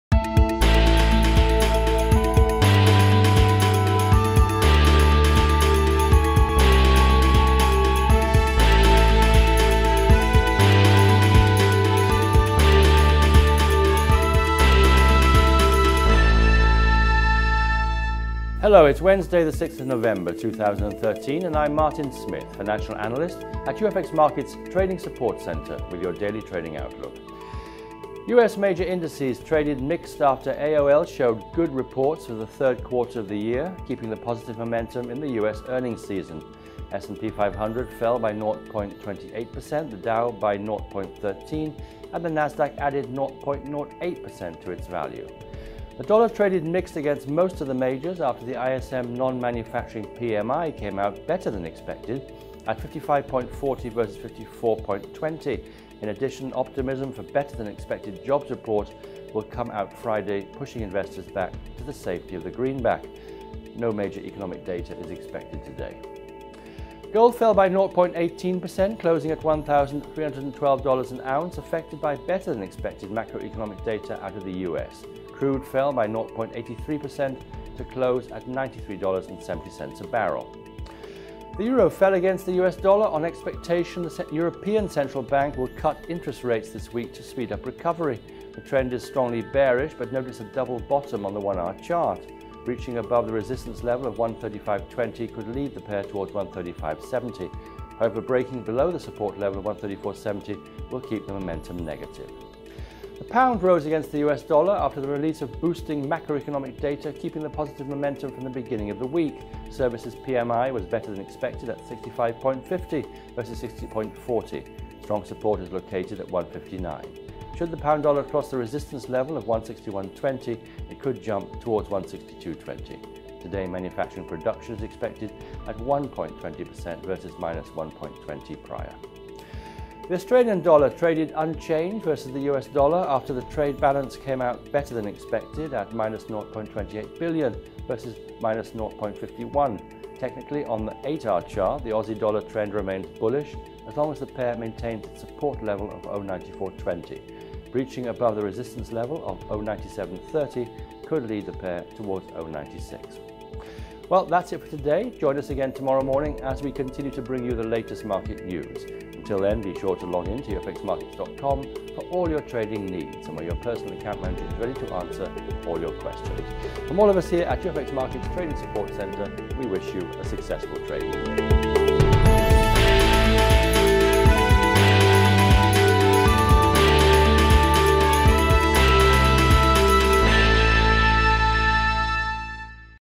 UFXMarkets Daily Forex Currency Trading News 06-November-2013
U.S Major Indices traded mixed after AOL showed good reports for the third quarter of the year, keeping the positive momentum in the U.S earnings session. S&P 500 fell by 0.28%, Dow Jones declined by 0.13% and NASDAQ added 0.08% to its value.  The Dollar traded mixed against most of the majors after the ISM Non-Manufacturing PMI came out better than expected at 55.40 vs. 54.200. In addition, optimism for better than expected jobs report will come out Friday pushing investors back to the safety of the greenback. No major economic data is expected today.

Gold fell by 0.18% closing at 1,312$ an ounce affected by better than expected macro-economic data out of the U.S. Crude fell by 0.83% closing $93.70 a barrel.